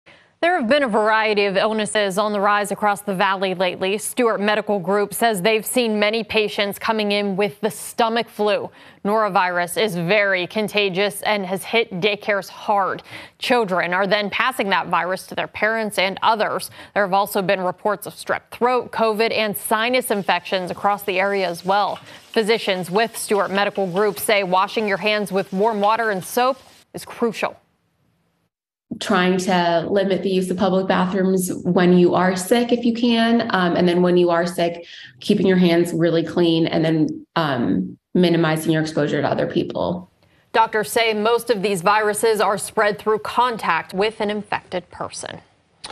Physician gives insight into illnesses on the rise
Physician Apurva Padubidri gives insight into illnesses on the rise